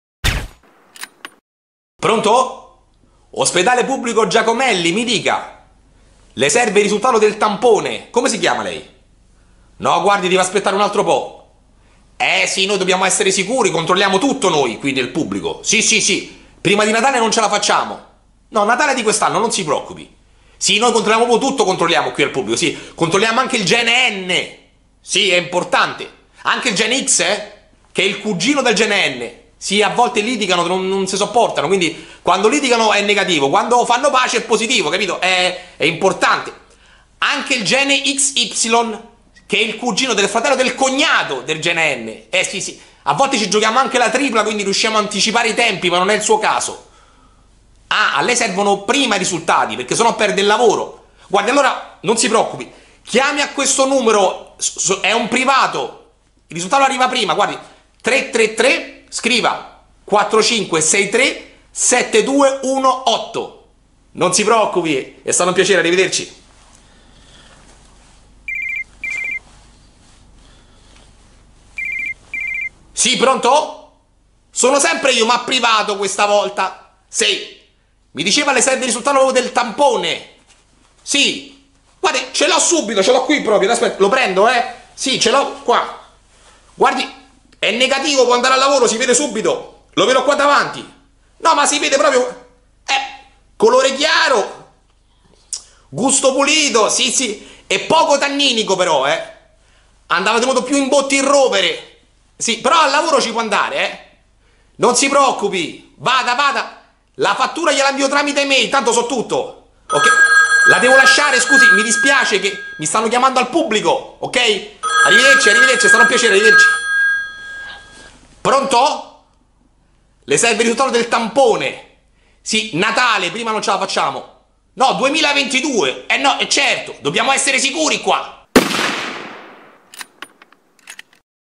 Analisi tampone PUBBLICO vs PRIVATO * Sergio Giuffrida SATIRA
Come funzionano le analisi dei tamponi? quale è la differenza nel PUBBLICO rispetto al PRIVATO?
I tempi sono gli stessi?

Interpretato da Sergio Giuffrida
Scritto da Gianluca Irti e Sergio Giuffrida